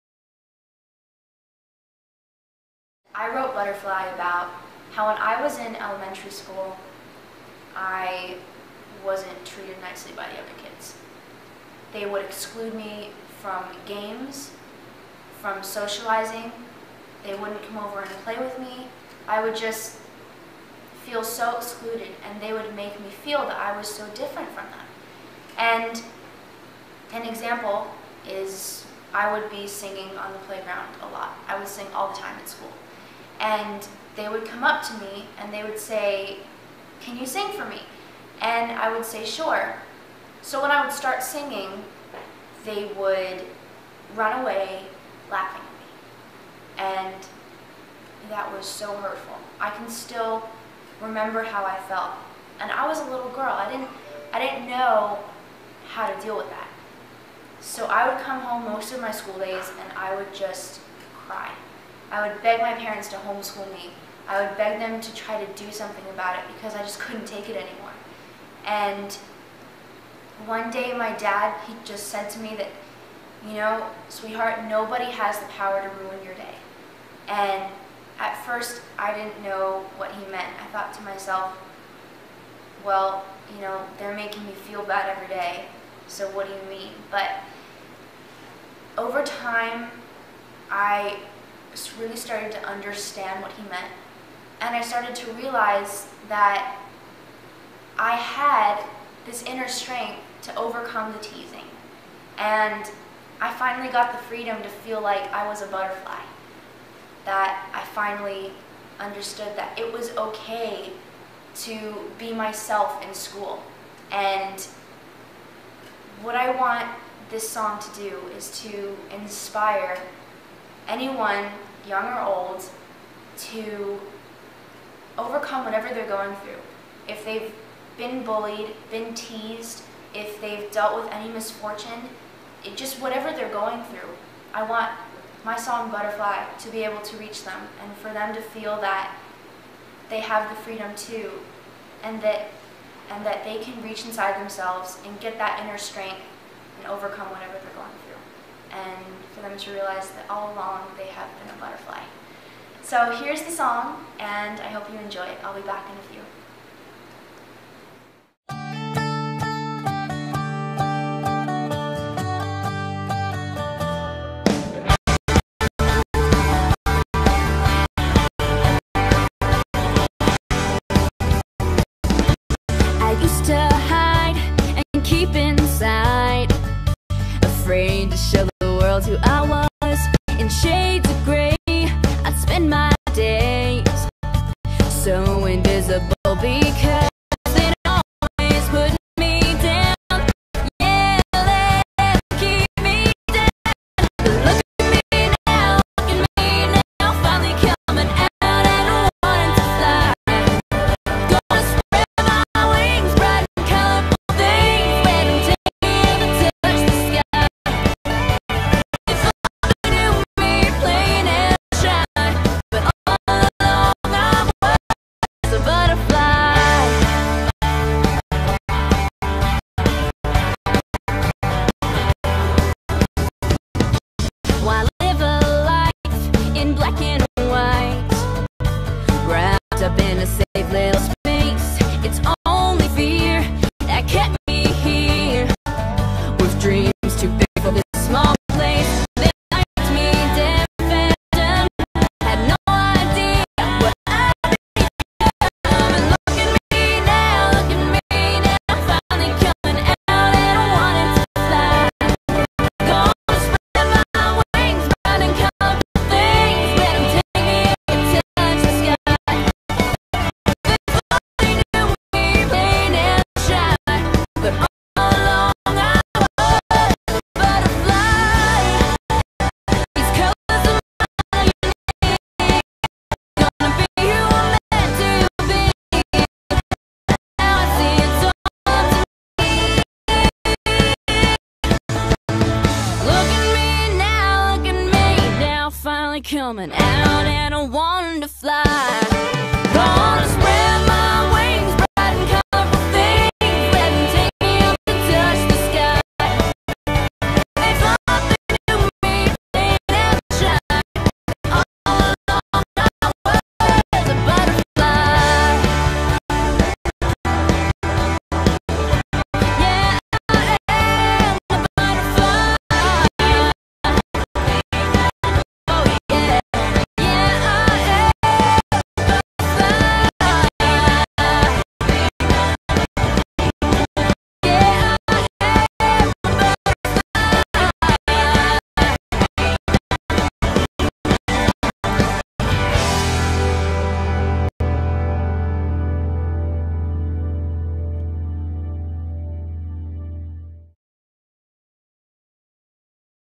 Global Women's Enpowerment Network
The Global Women's Empowerment Network (GWEN) is a global community that harnesses the power of technology to connect and empower those suffering from abuse.